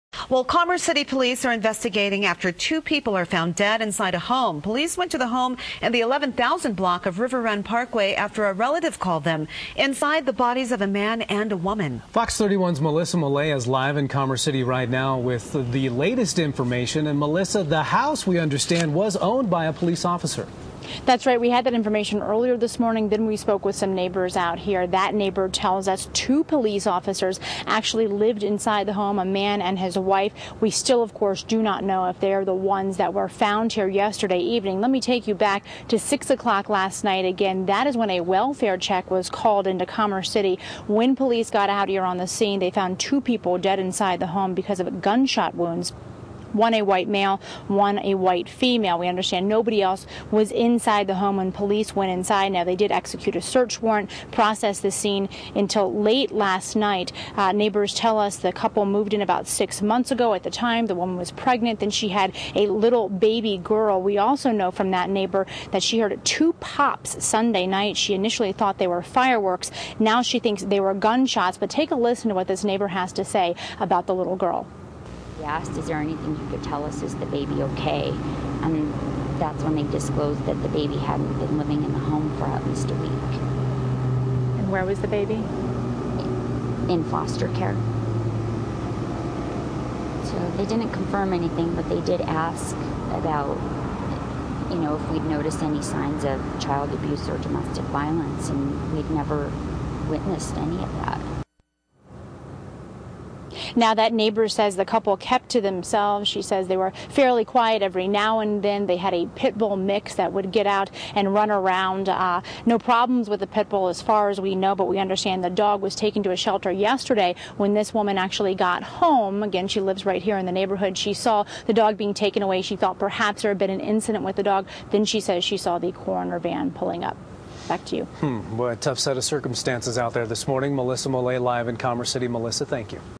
[CO] Officer-on-officer murder-suicide: The O'Shells [2]
LAKEWOOD POLICE DEPARTMENT
COLORADO
Officer William David O'Shell and his wife who was on maternity leave, Officer Tiffany Cuin-O'Shell.